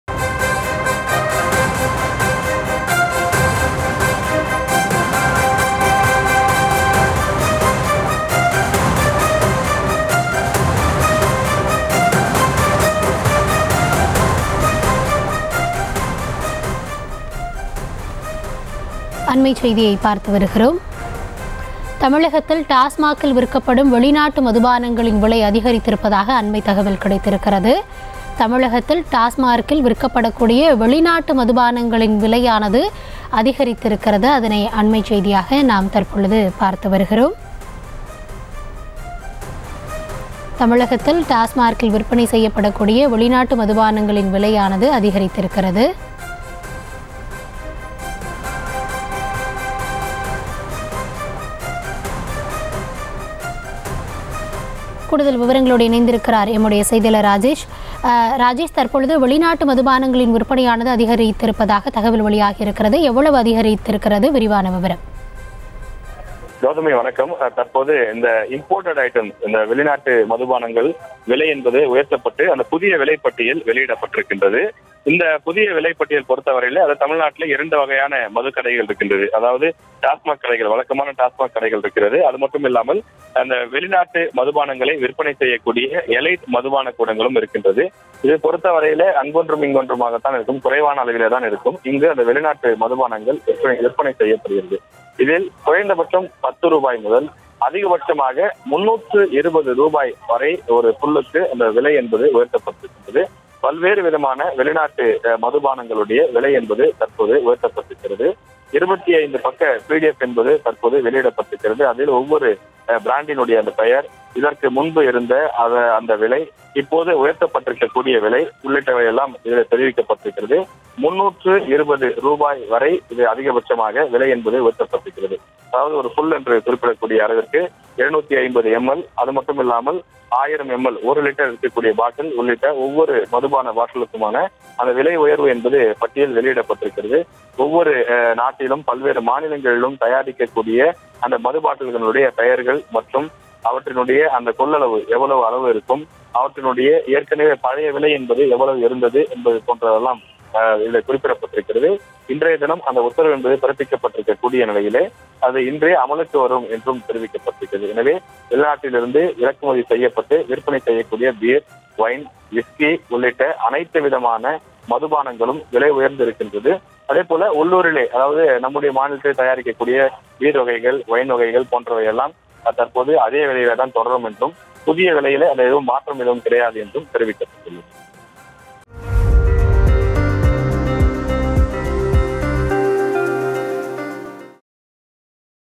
மதுப்பிரியர்களின் கவனதிற்கு.. வெளிநாட்டு மதுபான விலை அதிகரிப்பு | Foreign Liquor Price Hike
மதுப்பிரியர்களின் கவனதிற்கு.. வெளிநாட்டு மதுபான விலை அதிகரிப்பு | Foreign Liquor Price Hike | NewsTamil24x7